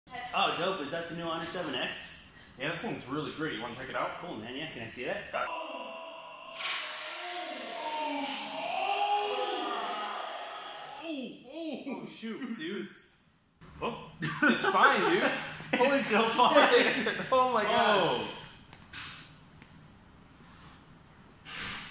Honor 7x Drop test
This exact device was dropped 3 times at our Miami Event and then two times to get this shot. Its perfectly fine and no cracks on the glass